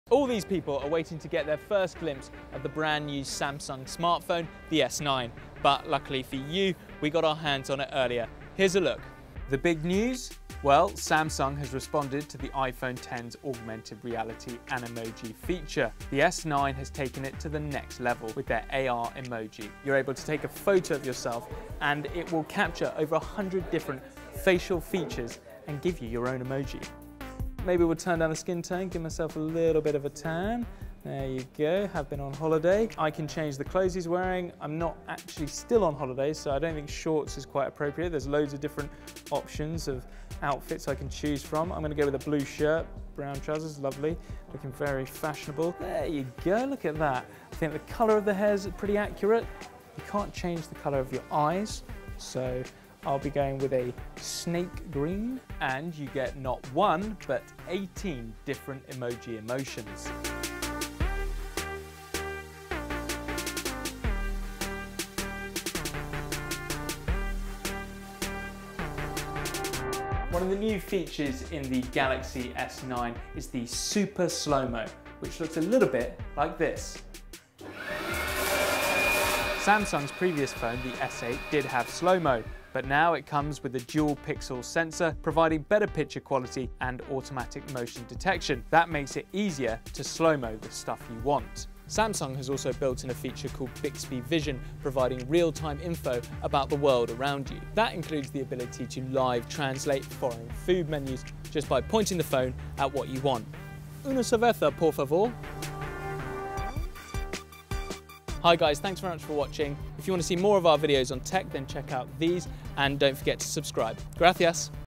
Samsung Galaxy S9 takes AR emojis to the next level | CNBC International
Samsung has responded to the iPhone X’s augmented reality Animoji feature by taking it to the next level with their own AR Emoji. CNBC's Tom Chitty reports from Mobile World Congress in Barcelona.